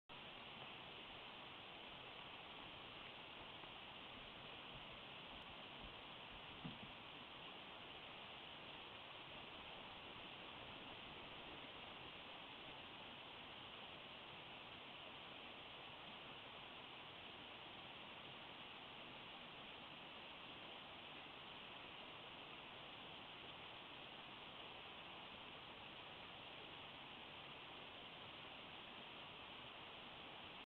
Grizzly Bears. 8/19/2016
Mama Griz & cub crossing the bridge.